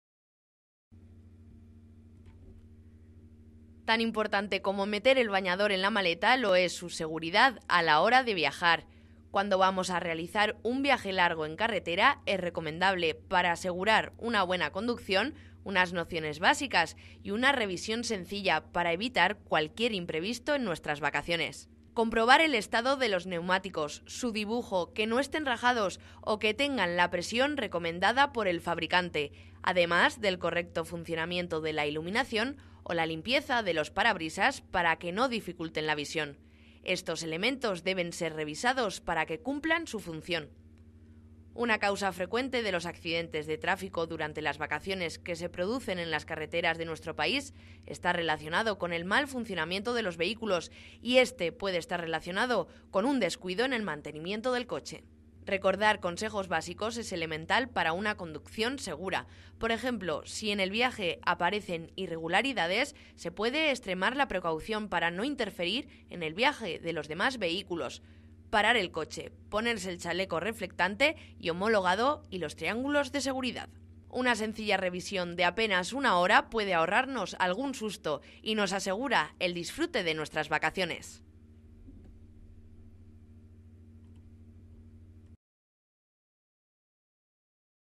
Si vas a viajar, revisa tu coche
En ocasiones, las vacaciones suponen largos desplazamientos. Por eso, antes de iniciar un viaje, conviene comprobar que todo está bien en el vechículo.